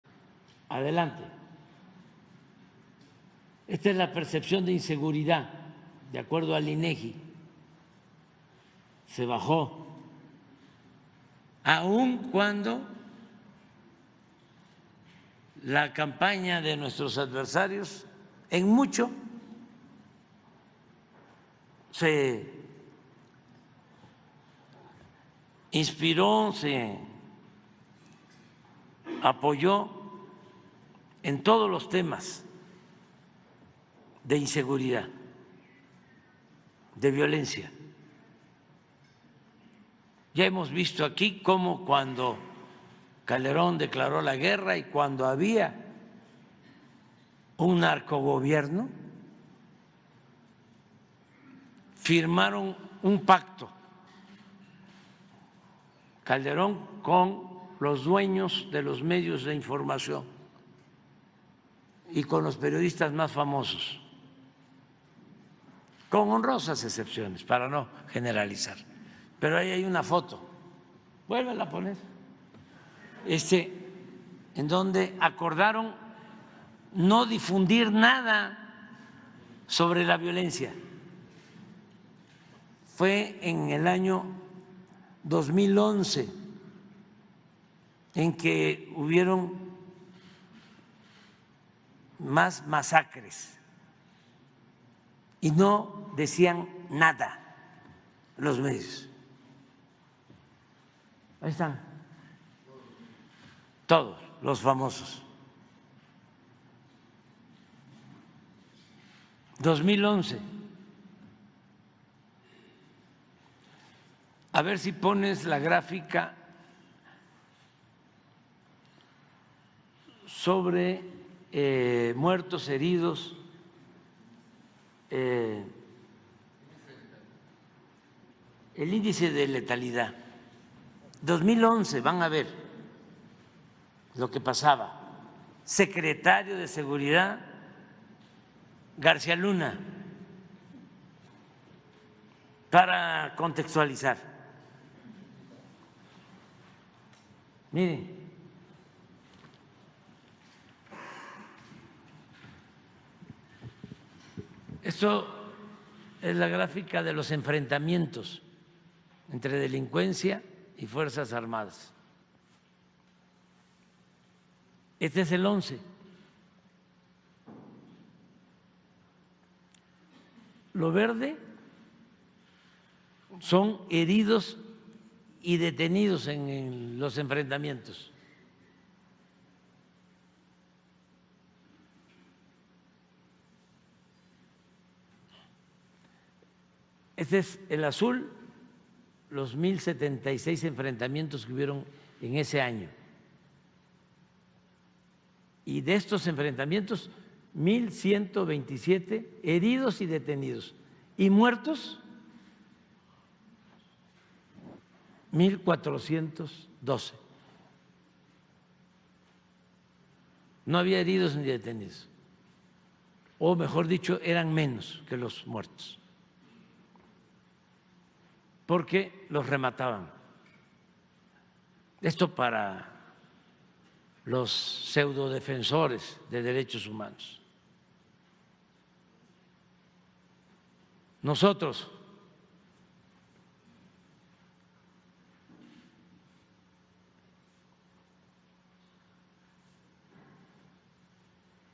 AMLO exhibe a GarciaLuna al hablar del índice de lεt4l1d4d en el país
El presidente LopezObrador exhibió una vez más a Genaro garcíaluna al hablar de índice de lεt4l1d4d en el país en el sexenio de borolas , y los comparó con su gobierno donde hubo una reducción significativa. Reconoció que a su gobierno le costó mucho detener el número de h0m1c1d10s y aprovechó para exhibir el pacto de silencio que hicieron los chayoteros y sus medios de porquería con el comandante